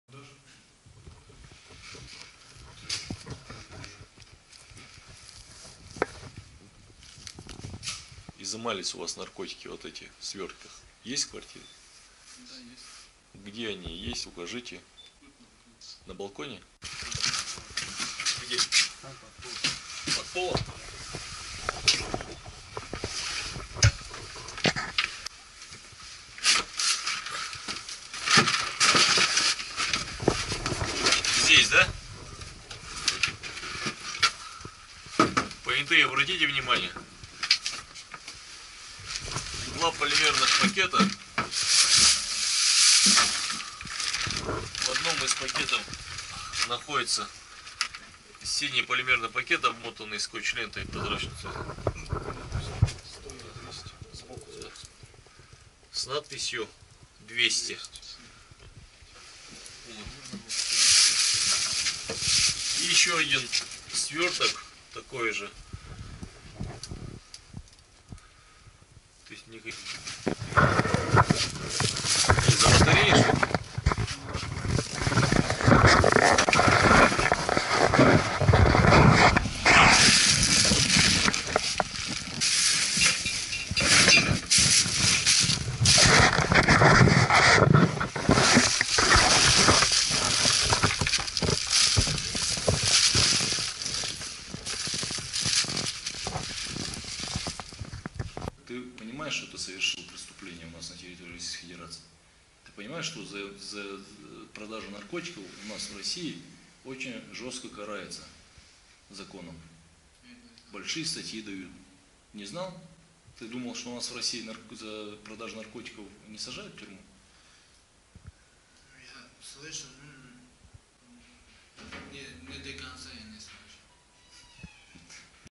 yarcube.ru — 3 кг героина изъято в Ярославской области
Дата: сентябрь-октябрь 2015 года.
Источник: ФЕДЕРАЛЬНАЯ СЛУЖБА ПО КОНТРОЛЮ ЗА ОБОРОТОМ НАРКОТИКОВ
Где: Ярославль и Ярославская область
Подробно: В ходе оперативной разработки наркополицейские установили личности членов этнической преступной группы, подозреваемых в сбыте крупных партий героина. Итогом кропотливой двухмесячной работы оперативников по документированию деятельности этой преступной группы стала операция по задержанию наркодилеров, проведенная с участием бойцов спецназа наркоконтроля «Гром». Торговцы смертью были задержаны в подъезде в момент, когда отправились делать закладки. Оба задержанных имеют спортивное прошлое: один боксер, другой – борец. В момент задержания они, используя свои спортивные навыки, пытались оказать сопротивление наркополицейским.